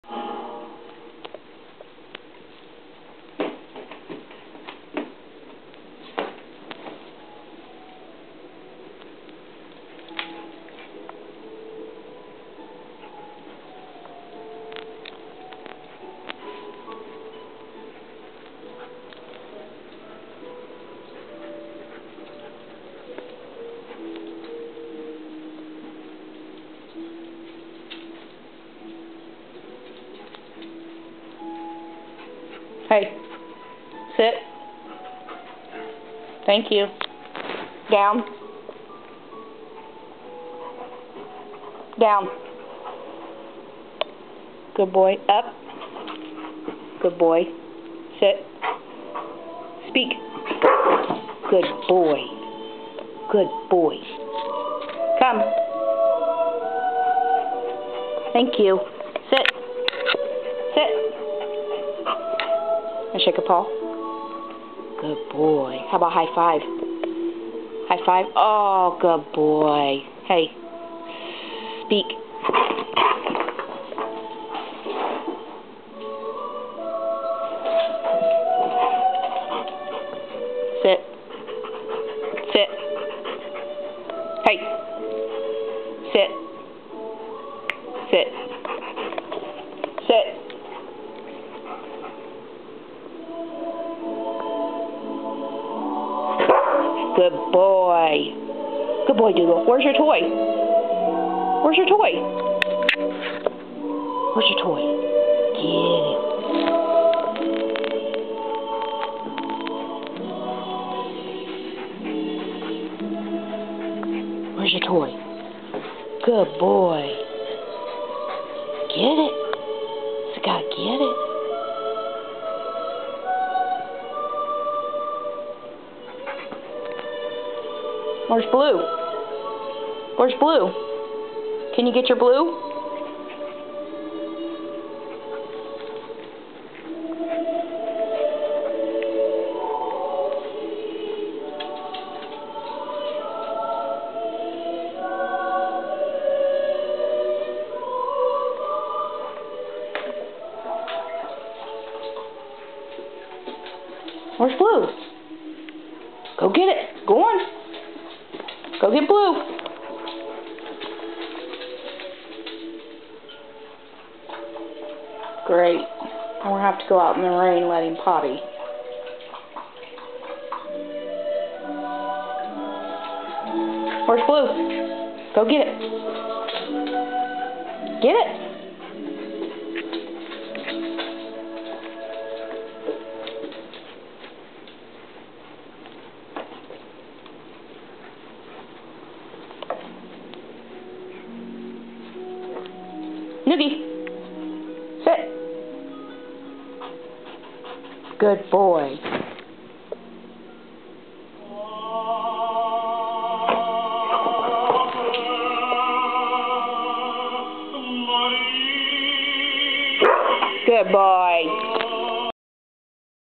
You're such a smart boy!
Dougal our smart pup practicing a few tricks!